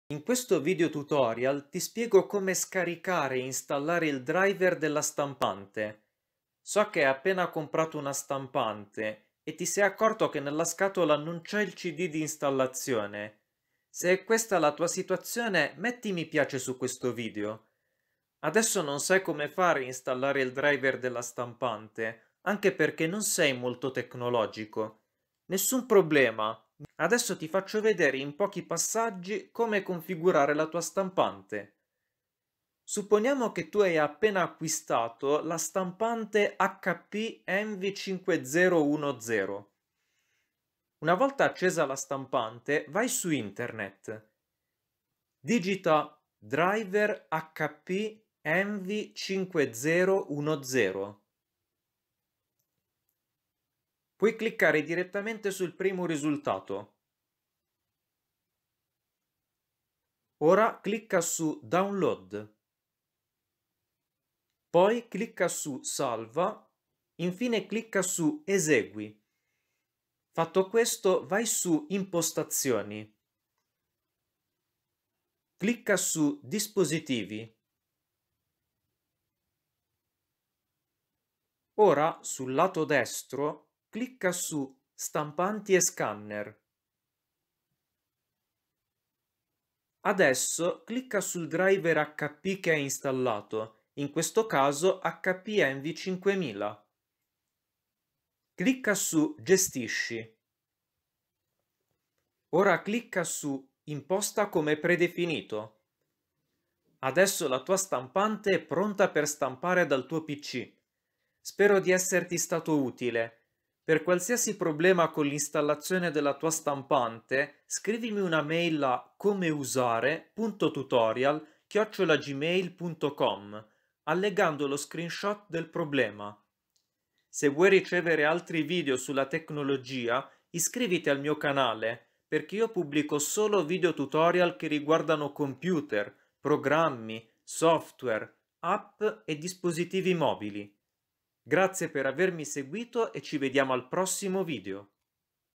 Come scaricare e installare il driver della stampante
"Non fanno più il cd di installazione della stampante!" Nessun problema! In pochi passaggi puoi scaricare il driver della tua stampante, installarla e impostarla come stampante predefinita.